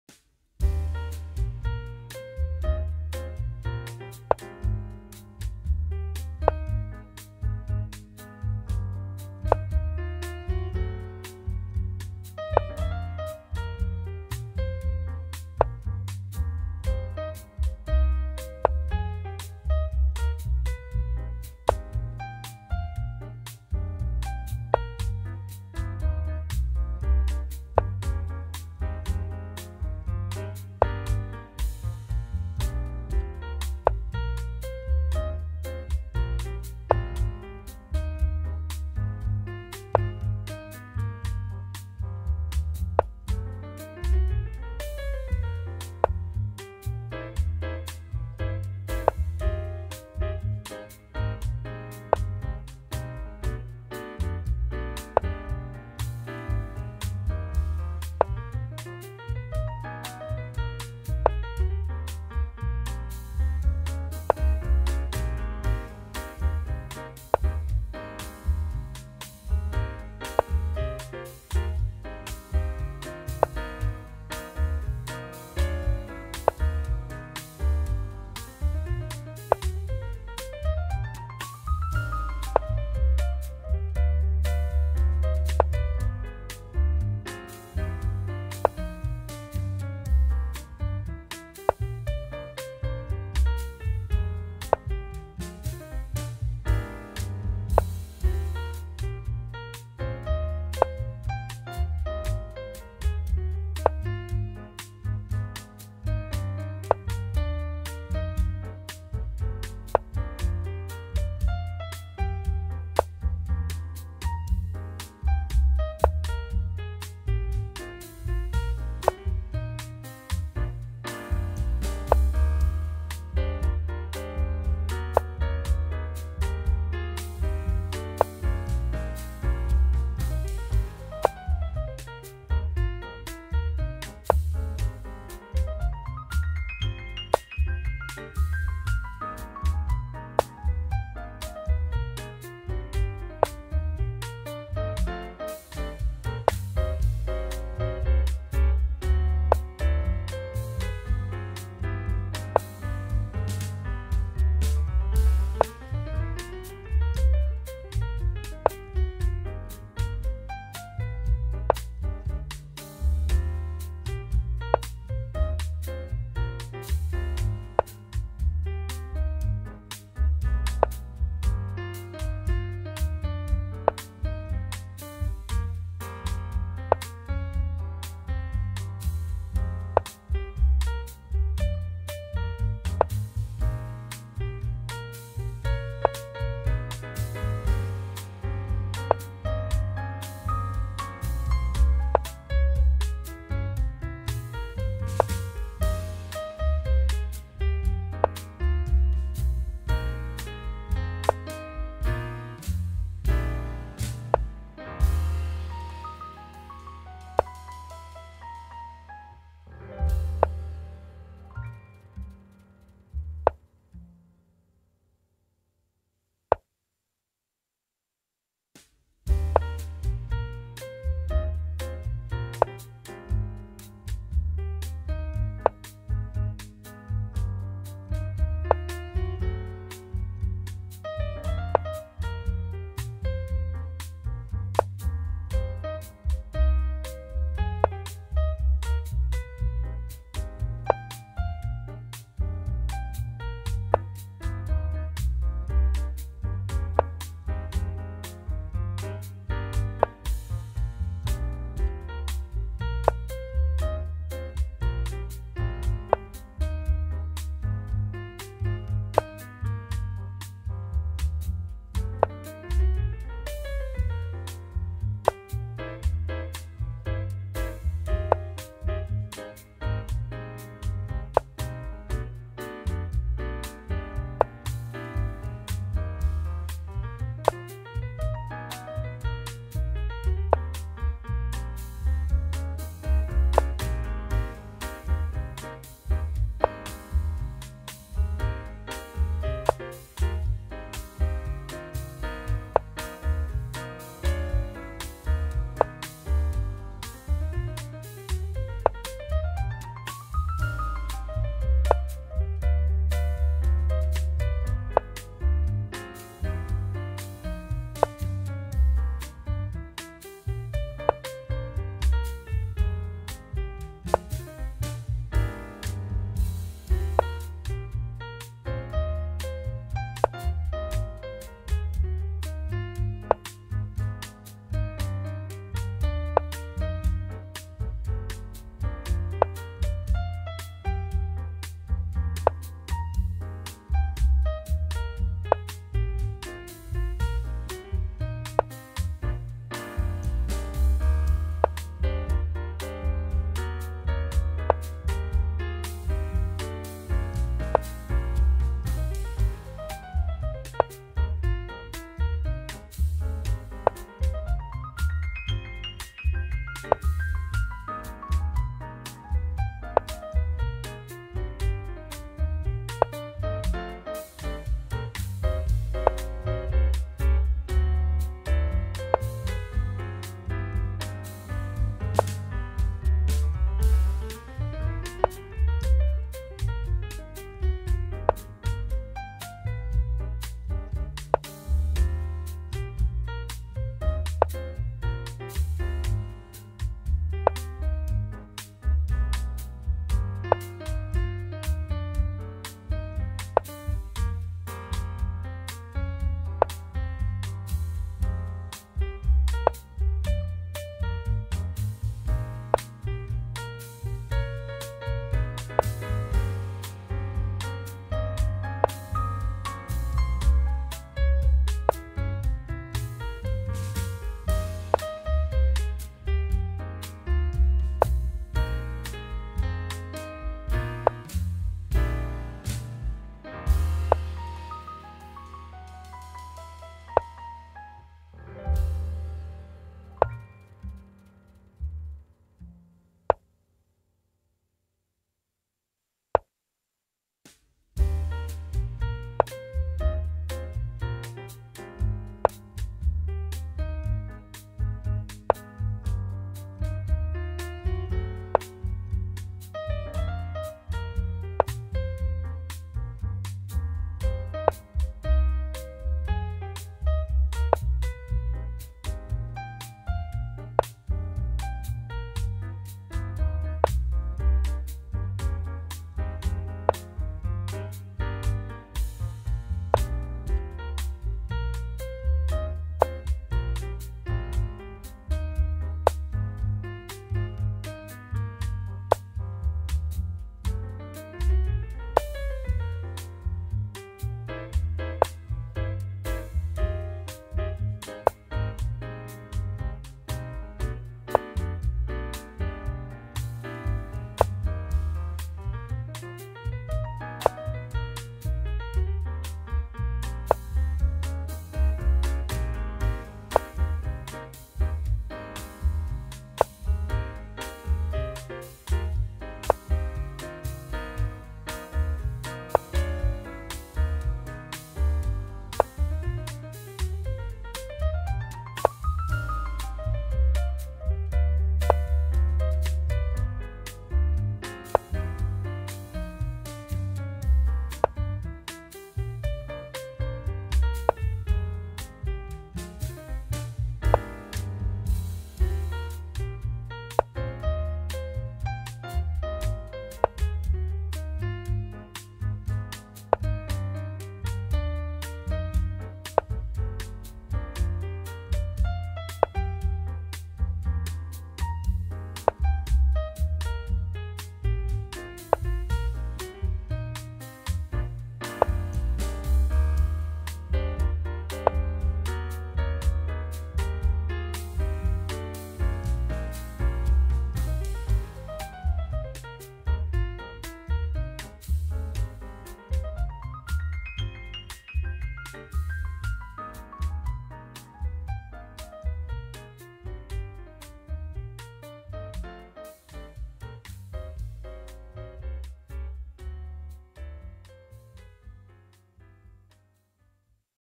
LCZero 0.30-dev+_782753 vs KomodoDragon 2909.00 | TCEC Season 22 - Subfinal Bonus | Round 5
LCZero 0.30-dev+_782753 vs KomodoDragon 2909.00
TCEC Season 22
Subfinal Bonus
Round 5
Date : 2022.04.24
Result 1-0

The Top Chess Engine Championship (TCEC) is the premier championship for chess software. Currently Season 22 is Superfinal between Stockfish and KomodoDragon.

If you like please comment !!